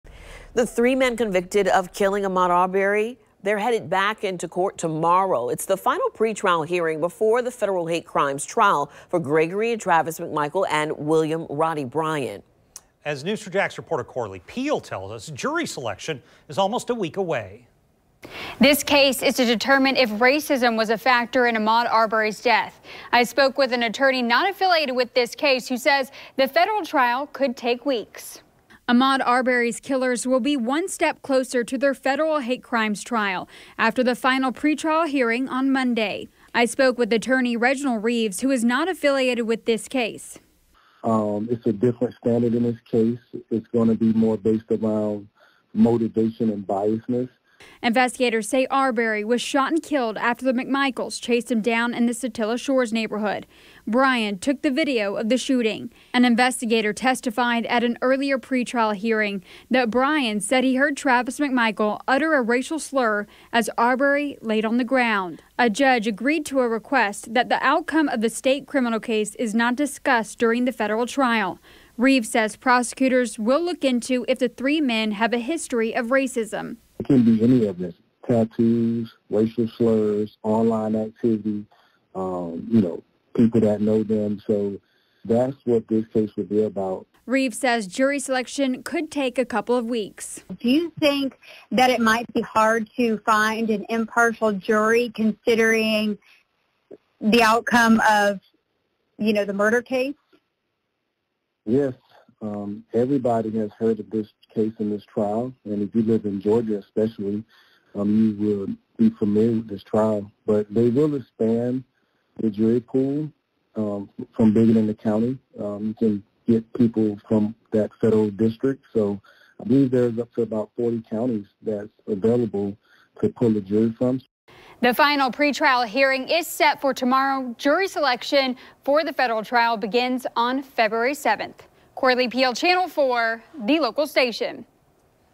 One more hearing for Ahmaud Arbery’s killers ahead of federal hate crimes trial
Ahmaud Arbery’s killers will be one step closer to their federal hate crimes trial after the final pretrial hearing on Monday.